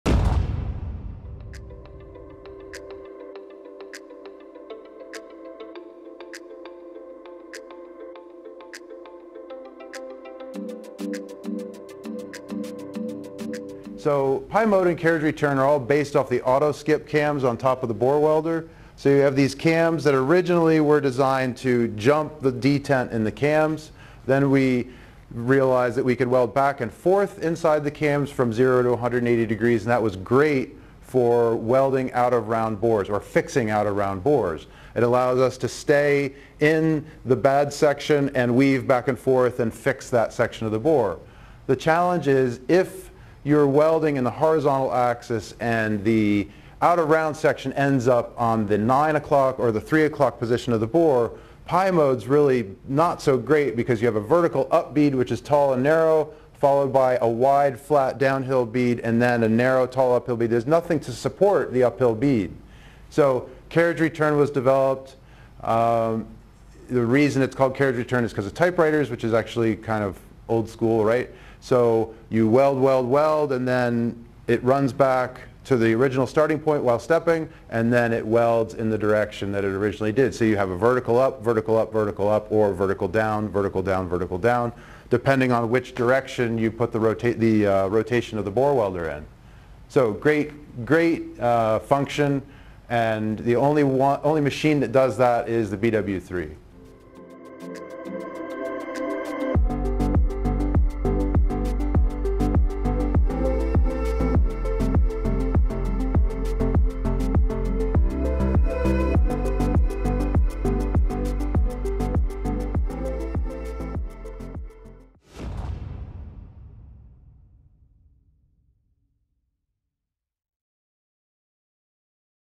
Auto Bore Welding: Difference between Pie Mode and Carriage return?
What is the difference between Pie Mode and Carriage return? Here’s a clip from the Bore Welding LIVE forum where Fitz Acheson answers this question.